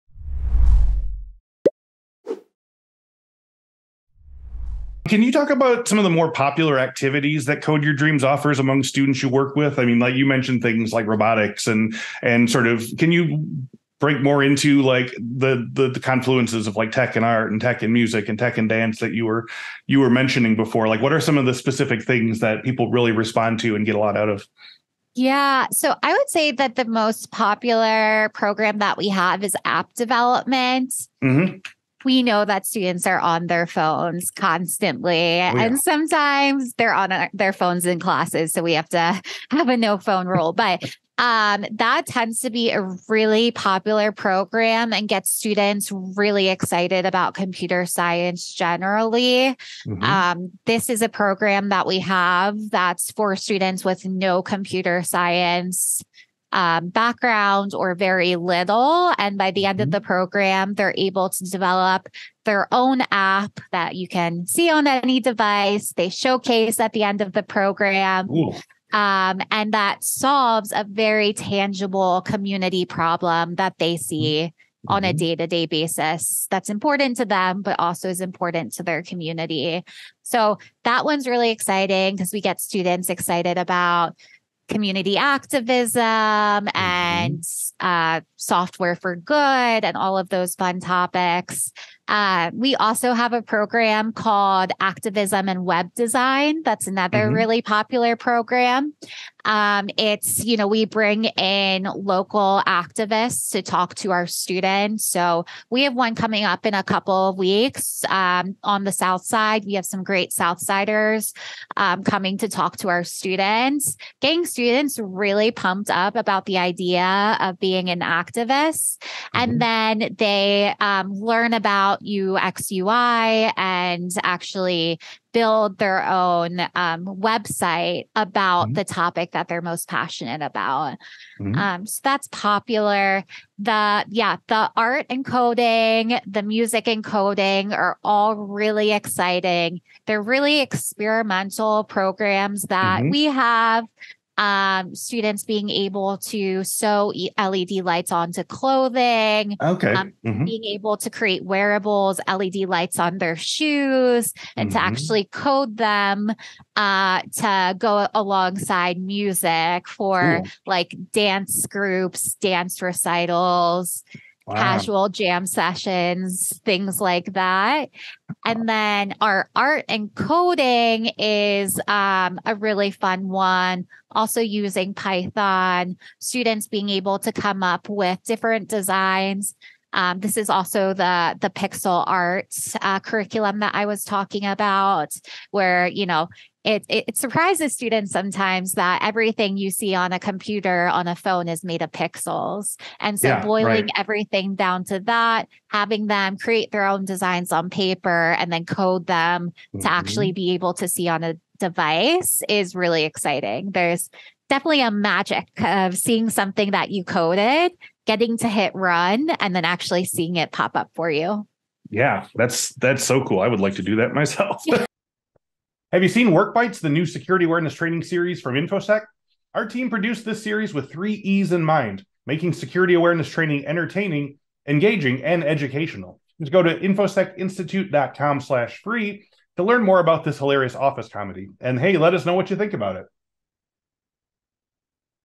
What do Code Your Dreams participants do? | Cyber Work Podcast
Brianne Caplan is the founder and executive director of Code Your Dreams, a non-profit that brings knowledge, accessibility and excitement about programming and tech to learners from age 5 to adulthood in underserved communities. Caplan talks about all the cool things Code Your Dreams participants get to do, including app development, activism and coding and projects that combine app programming with music, art and even dance!